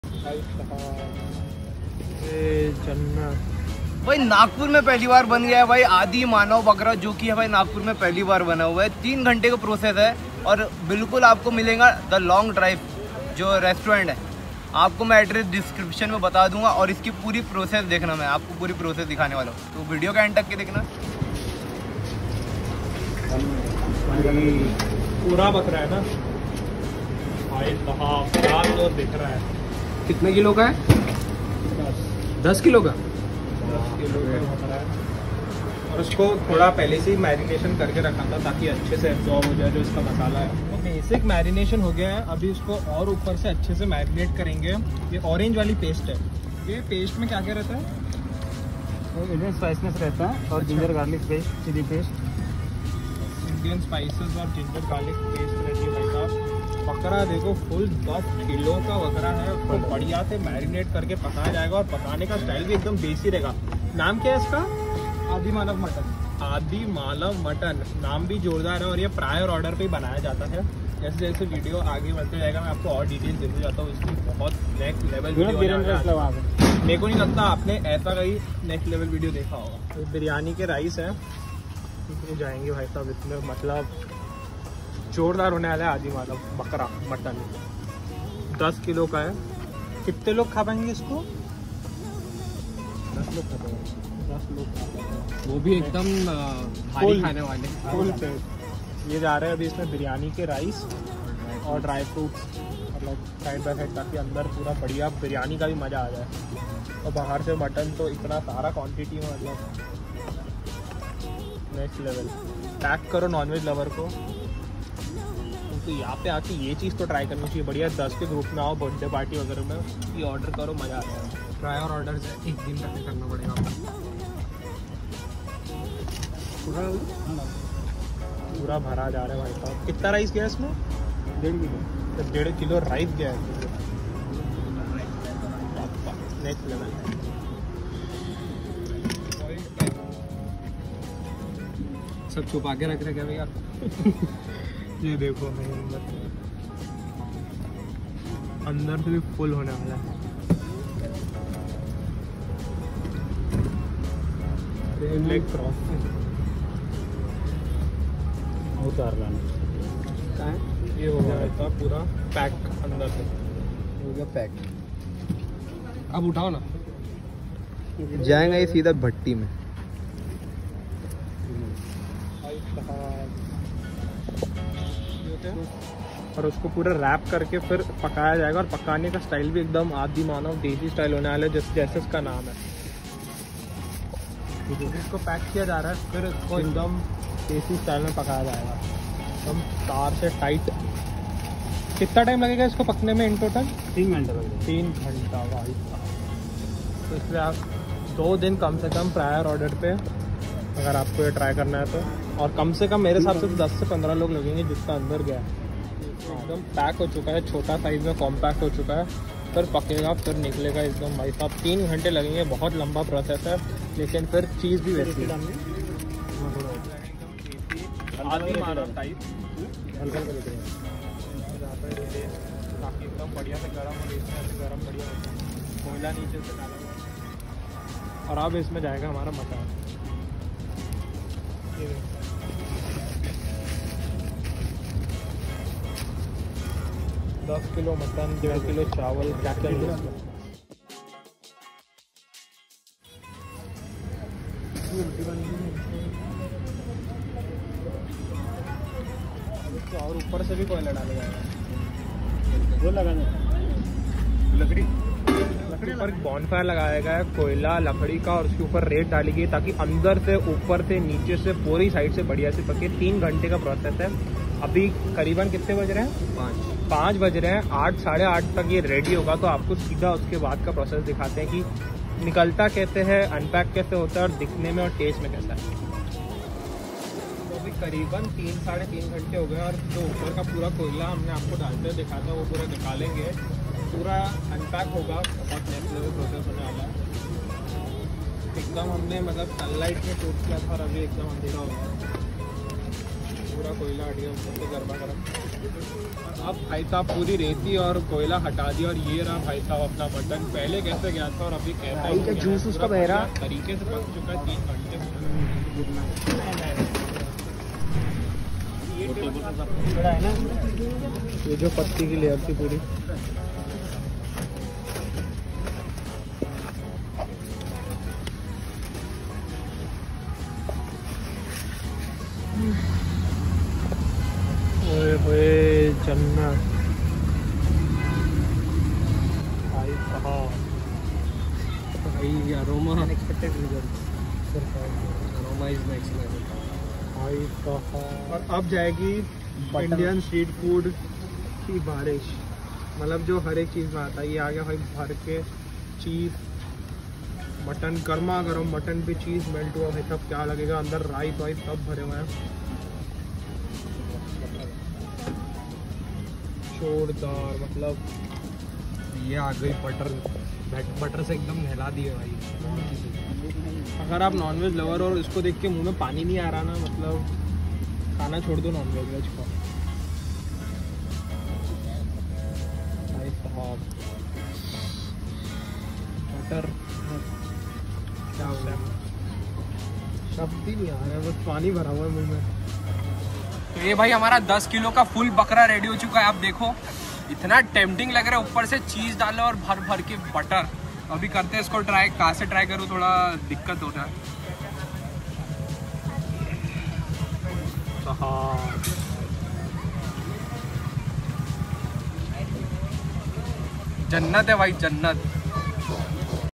Dubai style 40kg full bakra | non veg india | mutton recipe
Mutton recipe
Full goat preparation
Nagpur me pehli bar full bakra Dubai style me
Location : the long drive Restuarent Nagpur/ Wardha road.